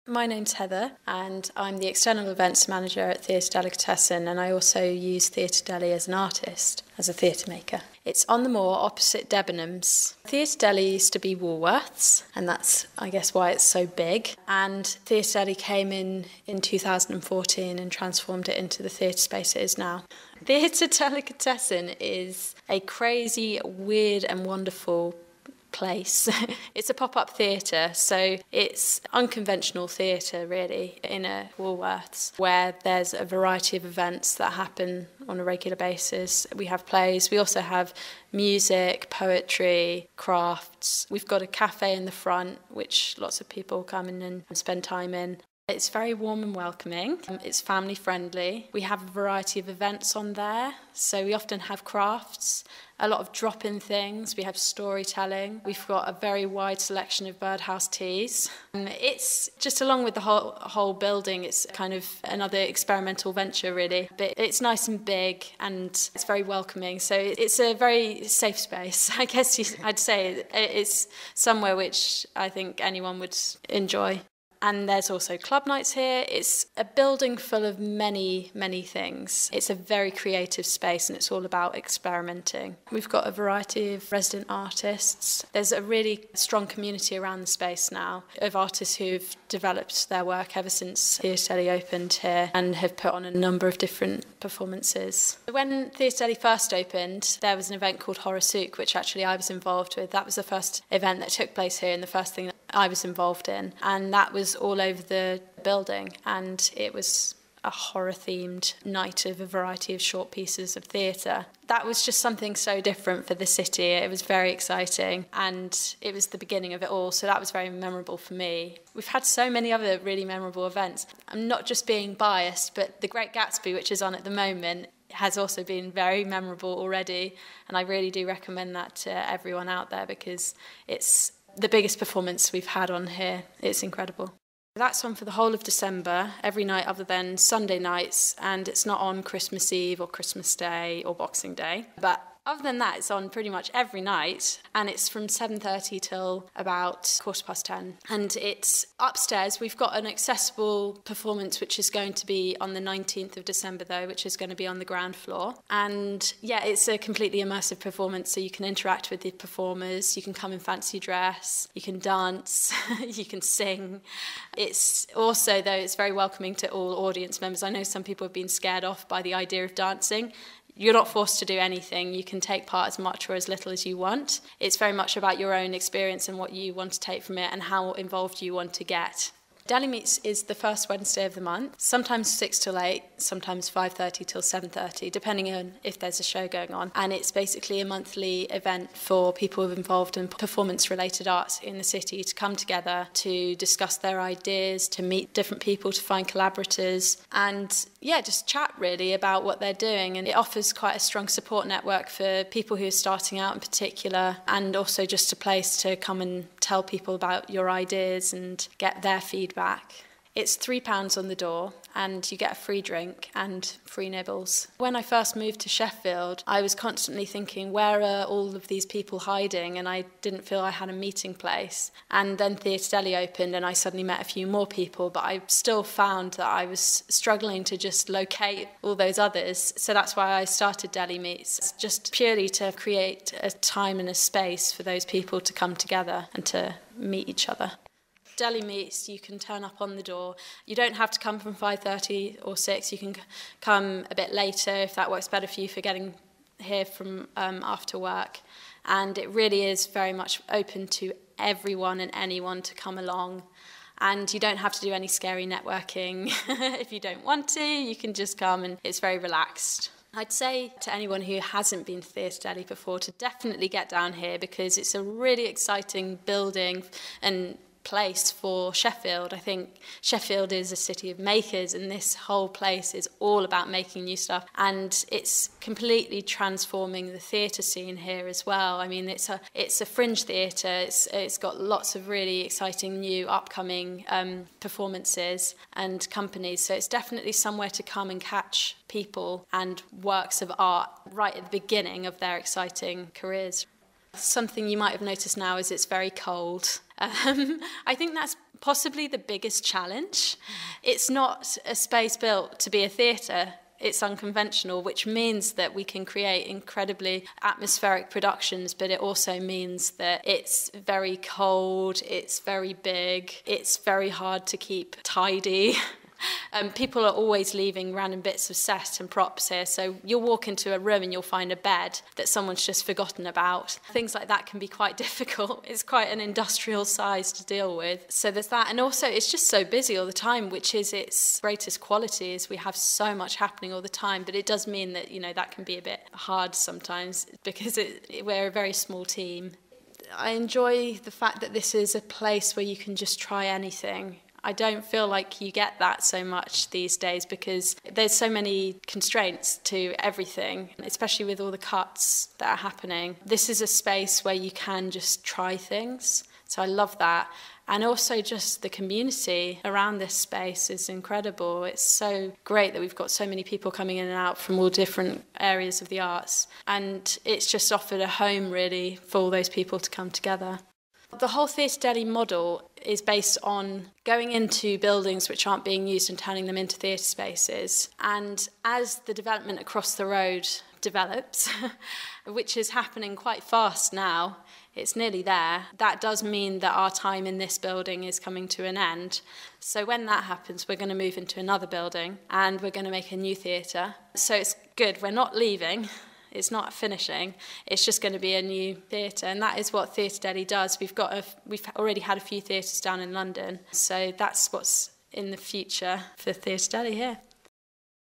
Heather Morgan from Theatre Deli, The Moor, Sheffield
Our Sheffield Live Radio (93.2FM, interview with Heather Morgan from Theatre Deli, The Moor, Sheffield (the old Woolworths Building) on Deli Meets and The Great Gatsby, throughout December 2016